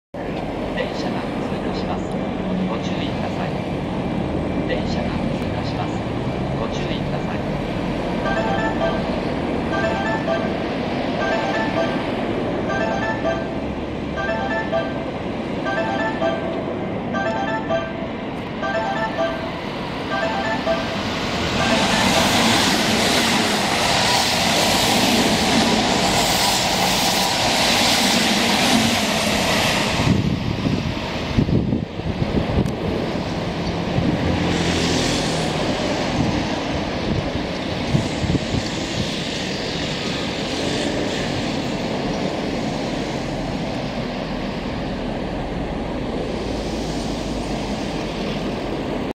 京阪本線 御殿山駅2番ホームを3000系特急が通過
2020年1月19日撮影

京阪本線　御殿山駅2番ホームを3000系特急が通過します。